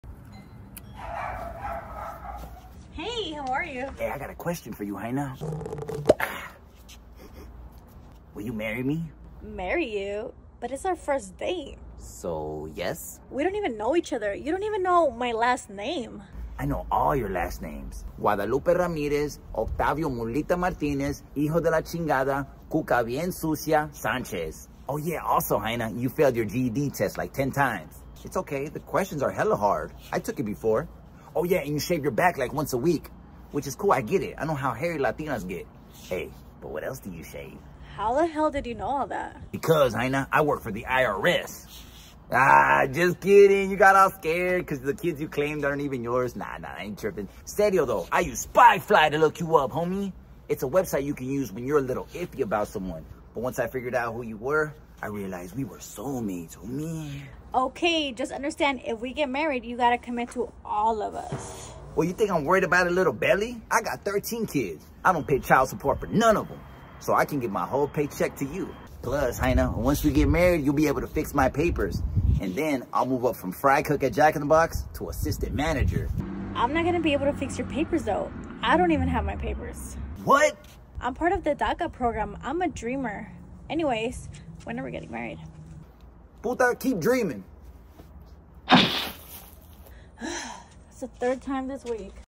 Why Using SpyFly on the First Date Is a Game-Changer!
Ever wondered what your date might be hiding? On first dates, be safe by using SpyFly to run a background report on him or her. With 1 in 3 people admitting to hiding key details about themselves online, it’s smart to know who you’re really meeting.